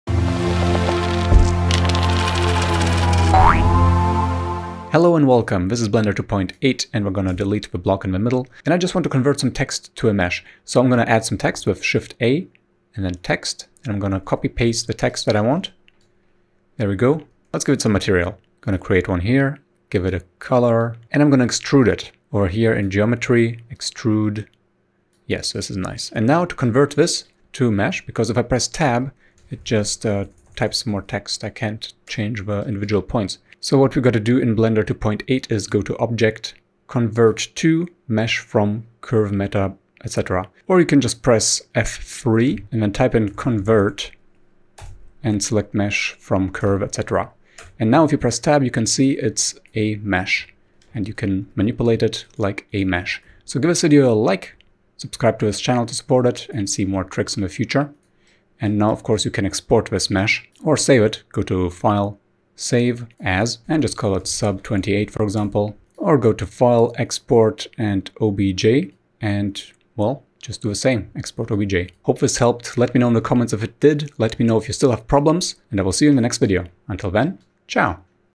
How to Convert Text to Mesh in Blender 2.8
How to convert curve or text to editable mesh in Blender 2.8 with few clicks, now that ALT+C doesn't work since after 2.79 unfortunately. In this video, we learn how to turn a any type of curve into a mesh in Blender 2.8, no matter if Eevee is turned on or not.